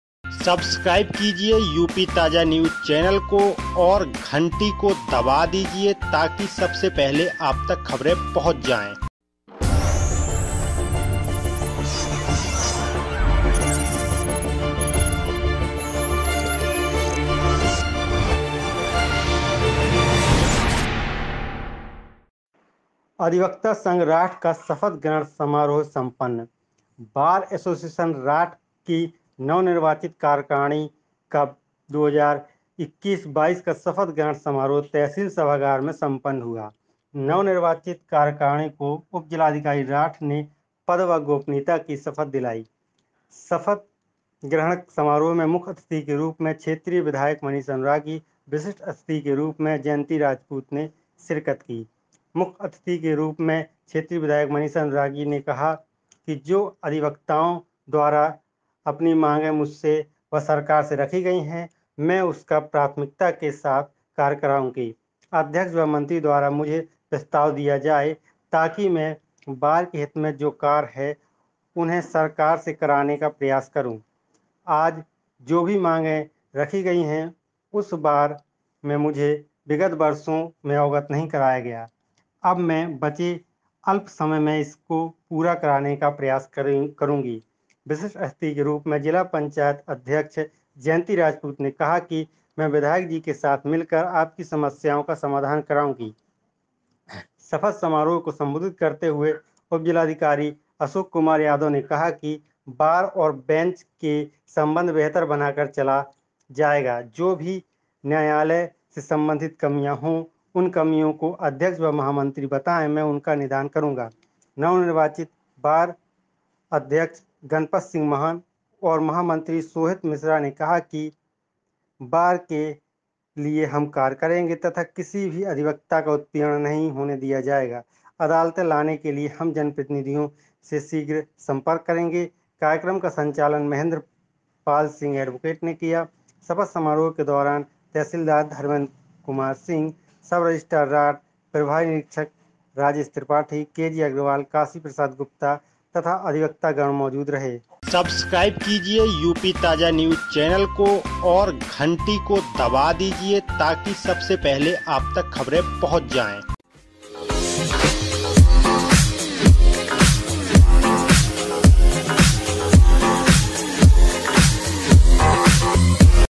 अधिवक्ता संघ राठ का शपथ ग्रहण समारोह सम्पन्न
राठ। बार एसोसिएशन राठ की नवनिर्वाचित कार्यकारिणी का 2021-22 का शपथ ग्रहण समारोह तहसील सभागार राठ में सम्पन्न हुआ।  नवनिर्वाचित कार्यकारिणी को उपजिलाधिकारी राठ ने पद व गोपनीयता की शपथ दिलायी। शपथ ग्रहण समारोह में मुख्य अतिथि के रूप में क्षेत्रीय विधायक मनीषा अनुरागी व विशिष्ठ अतिथि के रूप में जयंती राजपूत ने शिरकत की।
मुख्य अतिथि के रूप में क्षेत्रीय विधायक मनीषा अनुरागी ने कहा कि जो अधिवक्ताओं द्वारा अपनी मांगे मुझसे व सरकार से रखी गयीं हैं मैं उनका प्राथमिकता के साथ कार्य कराउंगी। अध्यक्ष व मंत्री द्वारा मुझे प्रस्ताव दिया जाये ताकि मैं बार के हित में जो कार्य हैं उन्हें सरकार से कराने का प्रयास करूं। आज जो भी मांगे रखी गयीं हैं उस बार में मुझे विगत वर्षांे में अवगत नहीं कराया गया। अब मैं अपने बचे अल्प समय में कार्य कराने भरपूर प्रयास करूंगी।
विशिष्ट अतिथि के रूप मंे जिला पंचायत अध्यक्ष जयंती राजपूत ने कहा कि मैं विधायक जी के साथ मिलकर आपकी समस्याओं का समाधान कराउंगी। शपथ समारोह को सम्बोधित करते हुये उपजिलाधिकारी अशोक कुमार यादव ने कहा कि बार और बेंच का संबंध बेहतर बनाकर चला जायेगा और जो भी न्यायालय से संबंधित कमियां हों उन कमियों केा अध्यक्ष व महामंत्री बतायें मैं उनका निदान करूंगा।
 नवनिर्वाचत बार अध्यक्ष गनपत सिंह महान और महामंत्री सोहित मिश्रा ने कहा कि बार के लिए हम कार्य करेंगे तथा किसी भी अधिवक्ता का उत्पीड़न नहीं होने दिया जायेगा, अदालते लाने के लिए हम जनप्रतिनिधियों से शीघ्र सम्पर्क करेंगे। कार्यक्रम का संचालन महेंद्रपाल सिंह एड. ने किया।  शपथ समारोह के दौरान तहसीलदार धर्मंेद्र कुमार सिंह, सब रजिस्टार राठ, प्रभारी निरीक्षक राजेश त्रिपाठी, के.जी. अग्रवाल, काशीप्रसाद गुप्ता तथा अधिवक्तागण मौजूद रहे।

संपादक
गनपत सिंह महान

UP TAAZA NEWS
TAJA NEWS
TAAZA NEWS
TAJA Khabar
taja khabar
taja news
hindi news
bundeli News
Bundelkhand news